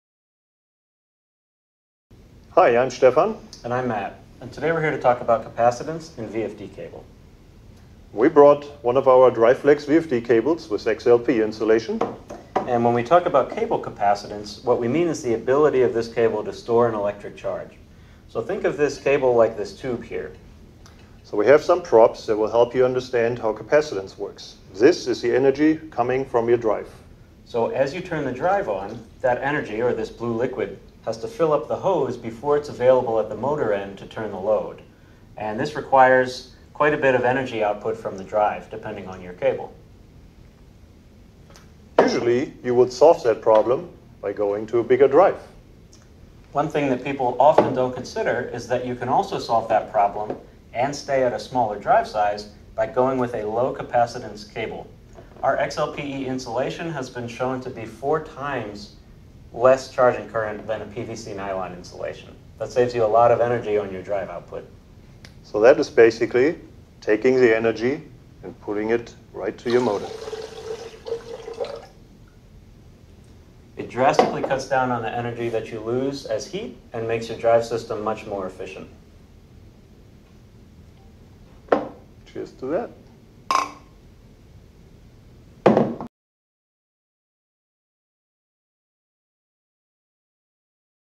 DRIVEFLEX®: Capacitance for VFD cables
How cable capacitance affects motor and VFD performance.